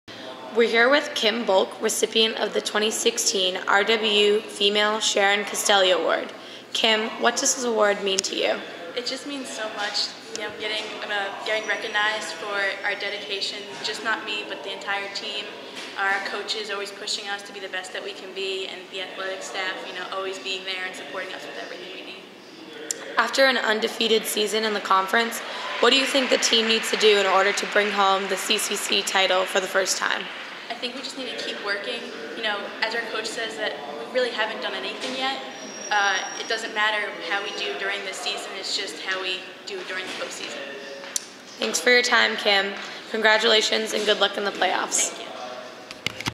2016 RWU Female Sharon Castelli Award Winner - Kimberly Bolk
Junior Women's Lacrosse player Kimberly Bolk talks about winning the 2016 RWU Female Sharon Castelli Award